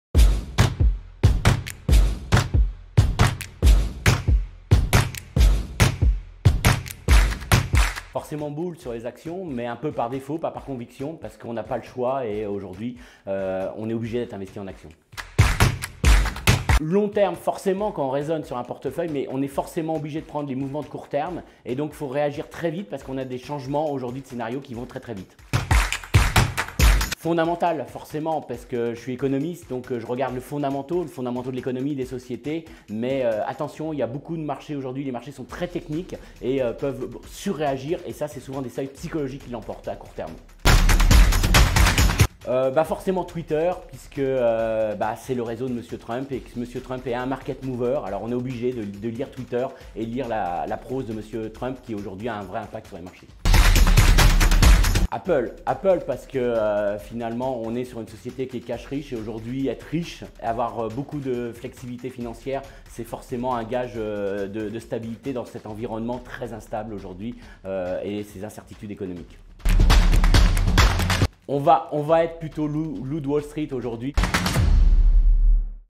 Fast & Curious - Chrisitan Parisot - IG France
Cette Nuit du trading était l'occasion de faire un Fast & Curious avec nos invité(e)s ✨
Le Fast & Curious - Chrisitan Parisot

76% des comptes d’investisseurs particuliers perdent de l’argent lorsqu’ils investissent sur les CFD avec IG*.

Le concept de Fast & Curious est directement inspiré de la séries du même nom publié par le magazine web Konbini